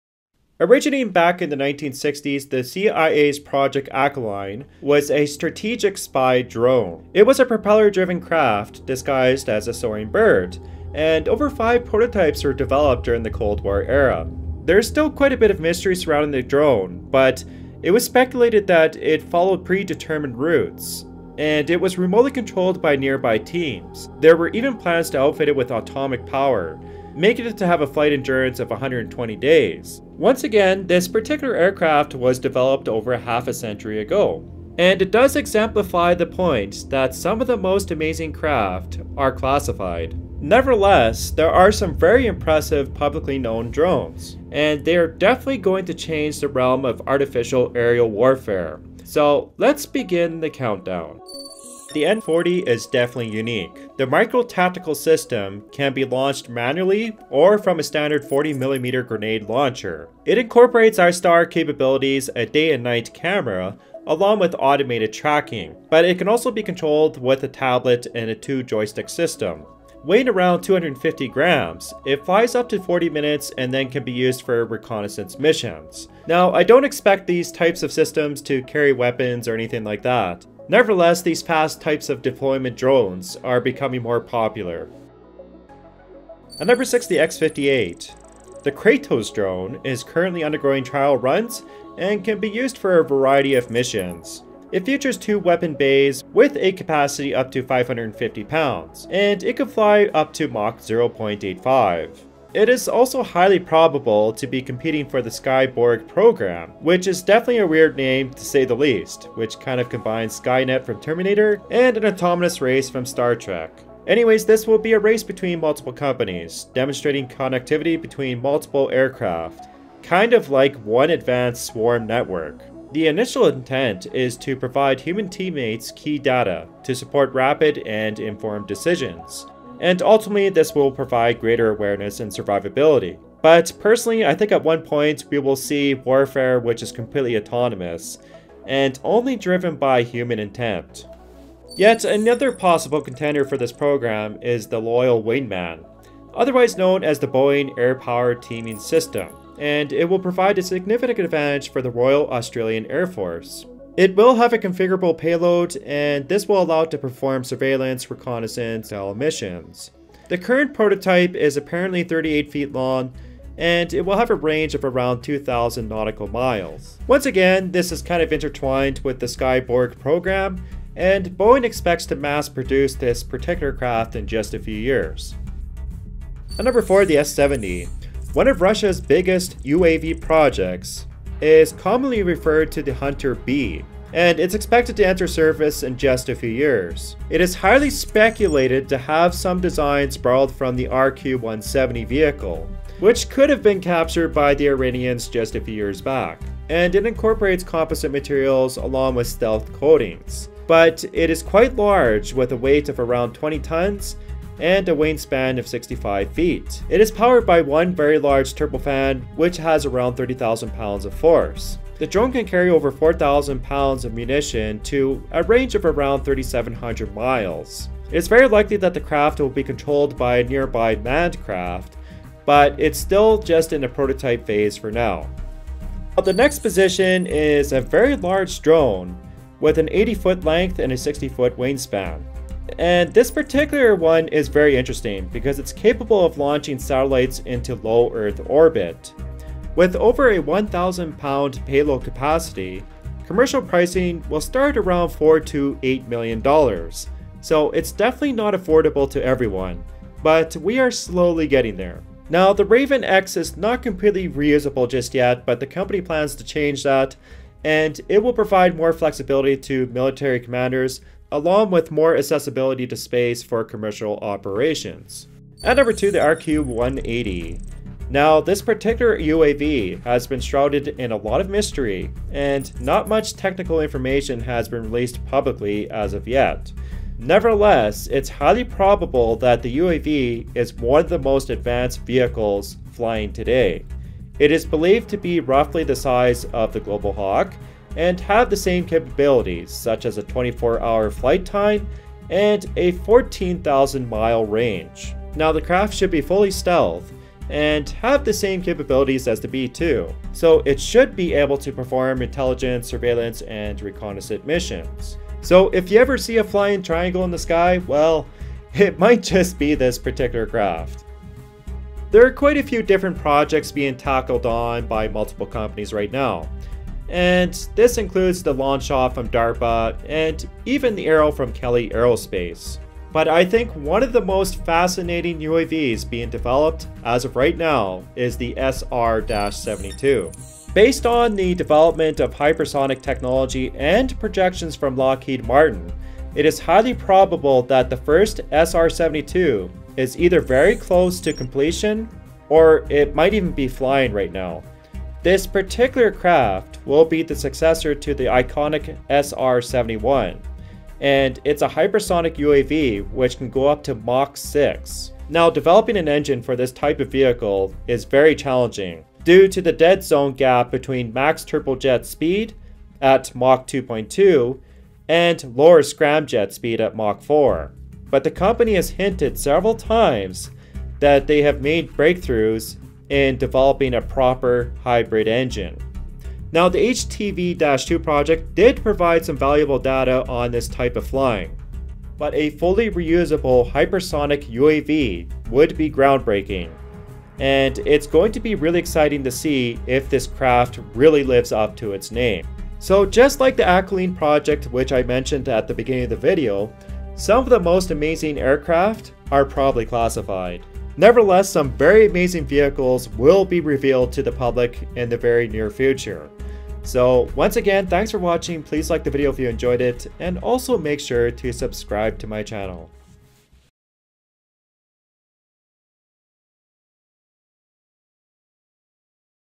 TOP 7 Deadliest UAVs In Development
We take a look at some of the most incredible drones and UAV's coming out right now!

7. Ninox 40

6. XQ-58 Valkyrie

5.  Loyal Wingman

4. S-70 Hunter B

3. Ravn X

2. RQ-180

1. SR-72